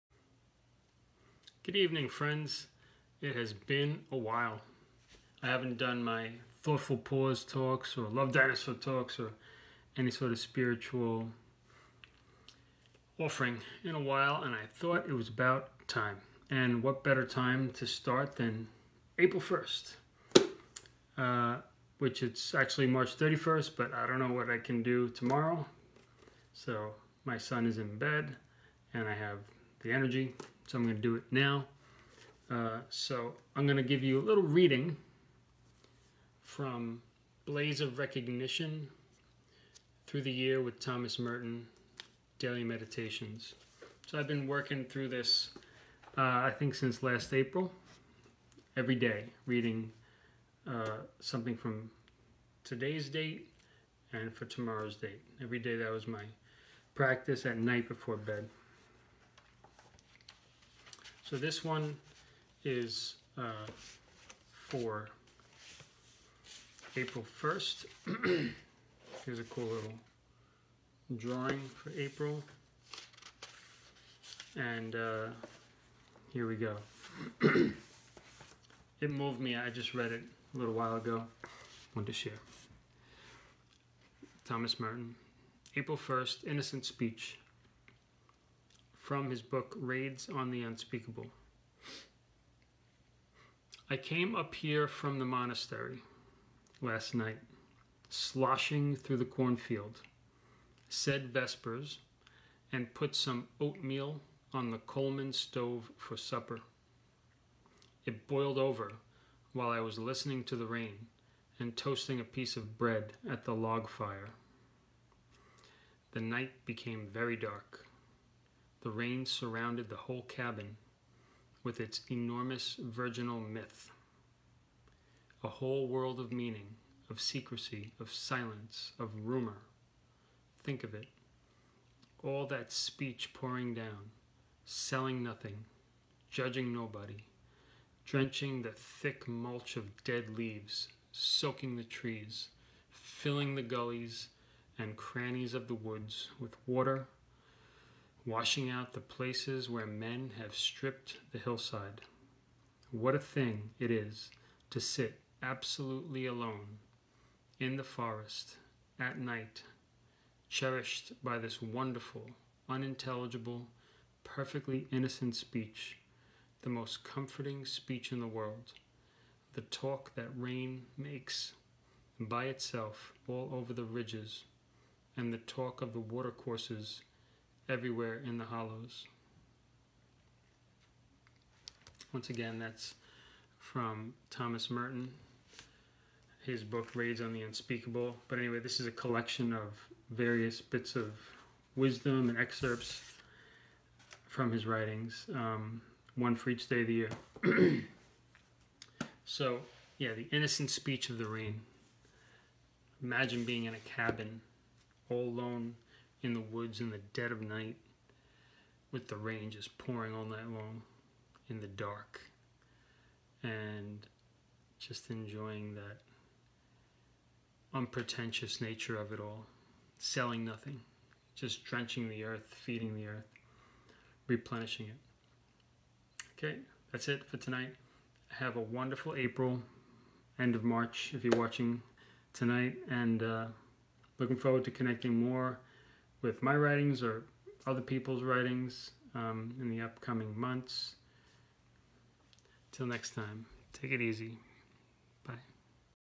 "Innocent Speech" - Thomas Merton
An excerpt from the writings of Thomas Merton.
From the book 'Blaze of Recognition - Through the Year with Thomas Merton: Daily Meditations' Selected and Edited by Thomas P. McDonnell